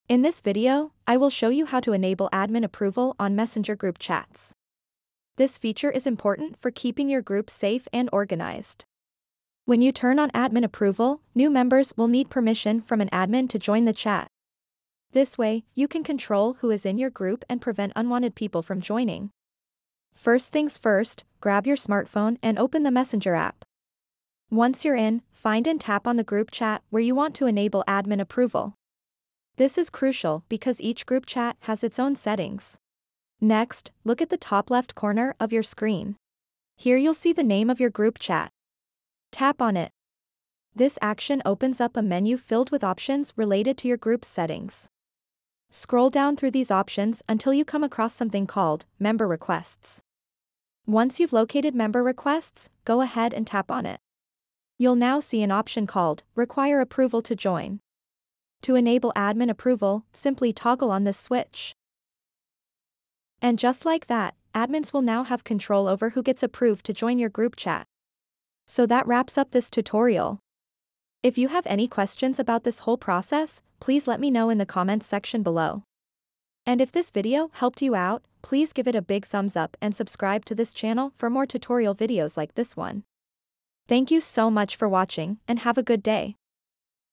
How To Enable Admin Approval On Messenger Group Chats
In this video, we will show you how to enable admin approval on Messenger group chats. This feature is useful for keeping your group organized and safe. When admin approval is turned on, only the admins can approve new members who want to join the chat. This means that you have more control over who is in your group.